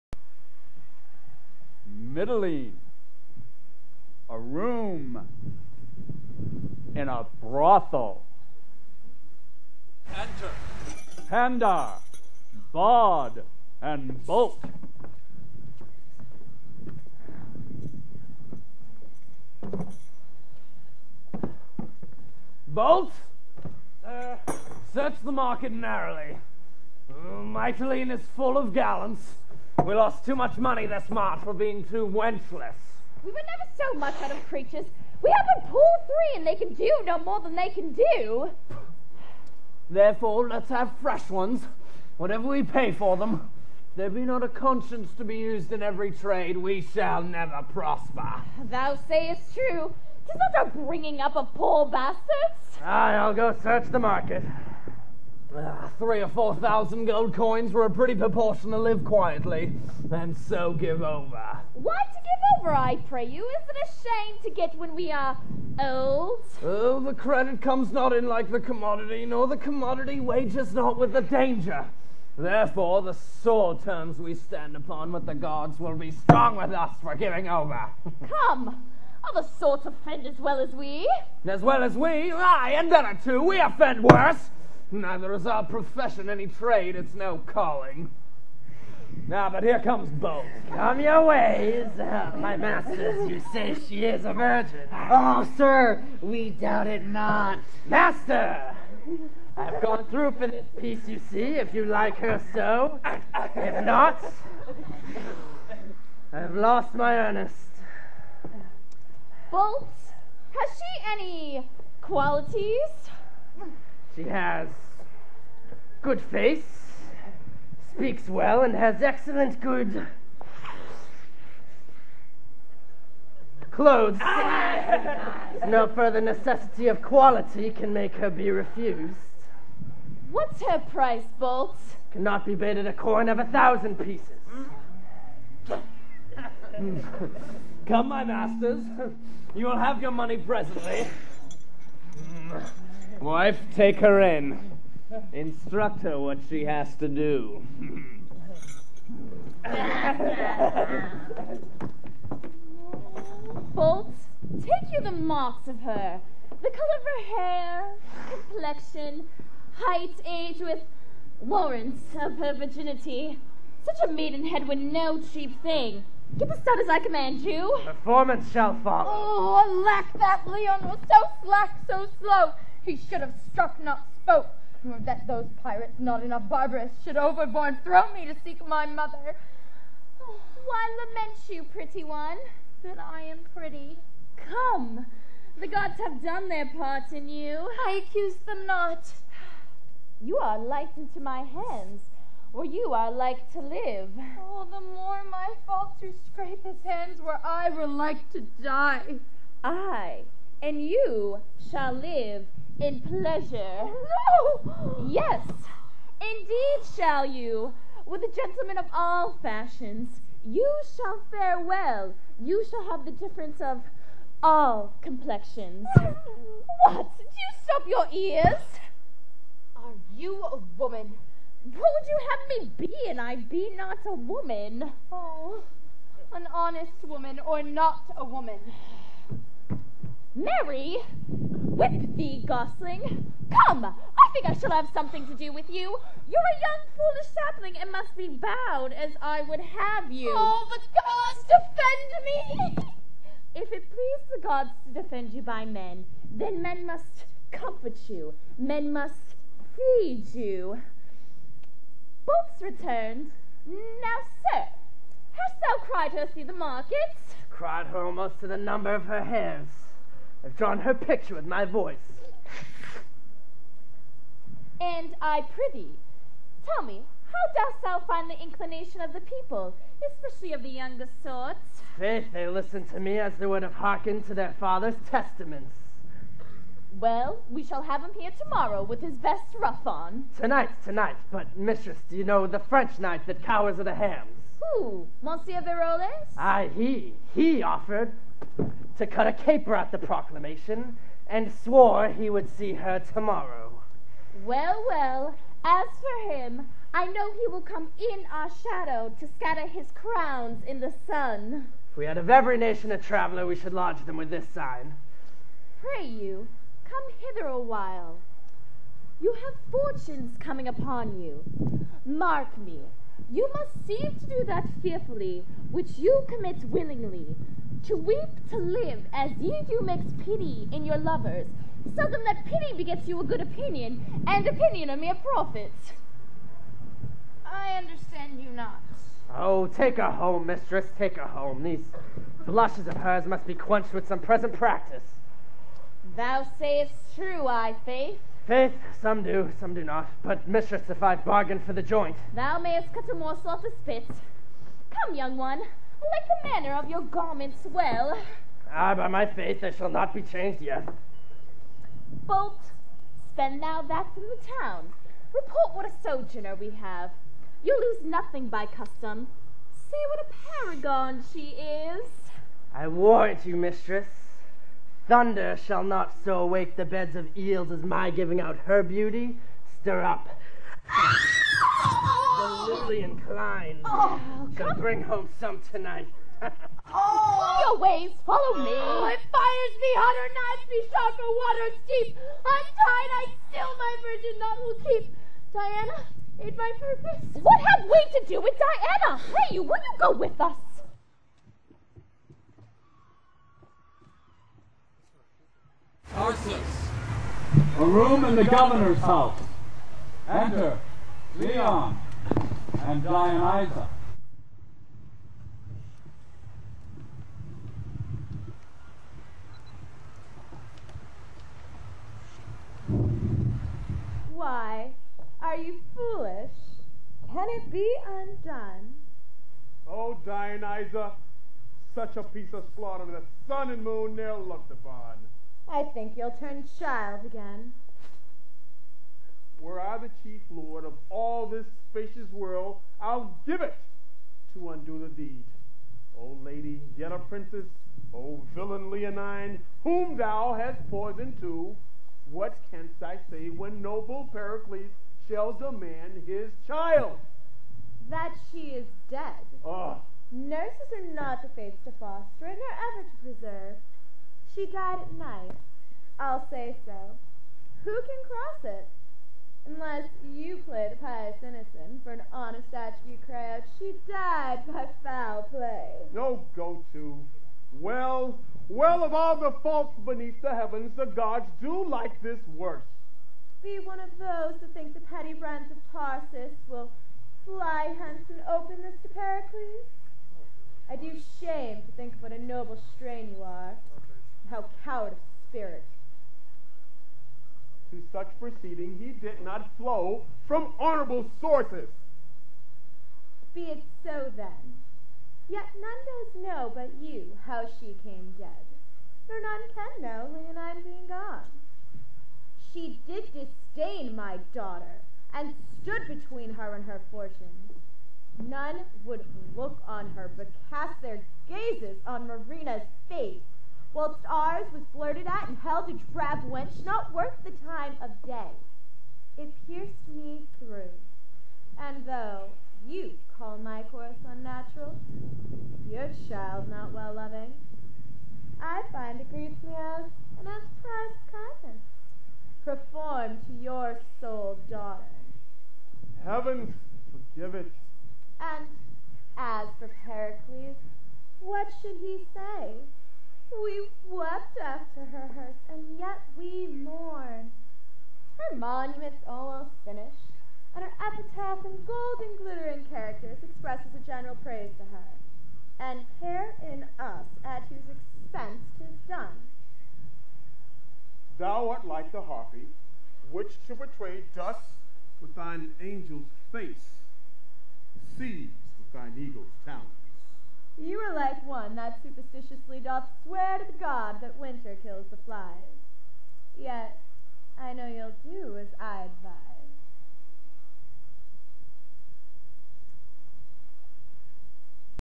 PERICLES ACT2 SCENE 4 & 5
Plymouth Community Theatre and Massasoit Community Theatre presents Pericles, Prince of Tyre, a play by William Shakespeare, directed by Dani Duggan, Technical Director/ Set Design Brian Willis and Asst. Technical Director Zach Vento. At the Plymouth Waterfront State Park Plymouth MA.
at the Plymouth Waterfront State Park.